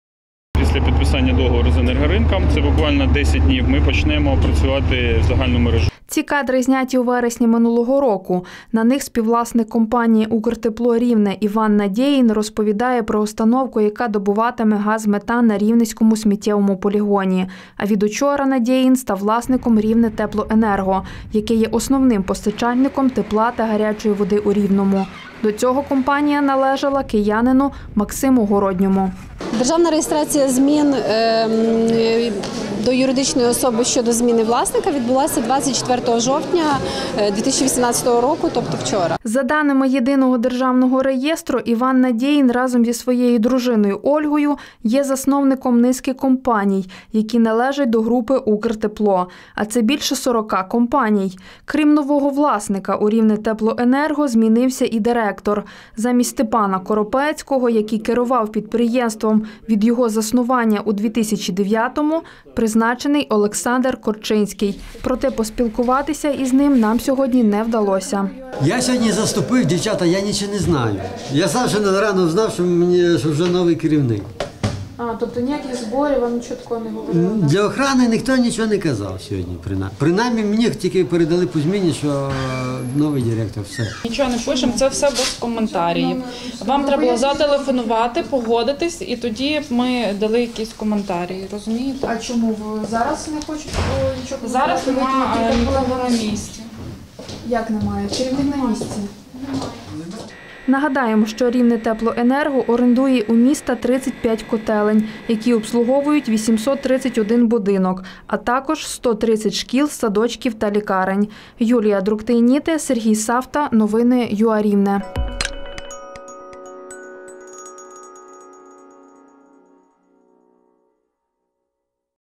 Сьогодні офіційно змінили директора "Рівнетеплоенерго"
Замість Степана Коропецького, призначено Олександра Корчинського.

А напередодні у ЦНАПі зареєстрували нового власника "Рівнетеплоенерго". Ним став Іван Надєїн. Про це розповіла державна реєстраторка юридичних осіб Рівненського ЦНАПу Ольга Грязнова.

За даними Єдиного державного реєстру, Іван Надеїн є співвласником компанії "УКРТЕПЛО РІВНЕ", яка з вересня минулого року почала видобувати газ метан на рівненському сміттєвому полігоні.

Деталі дізнавалася Юлія Друктейніте.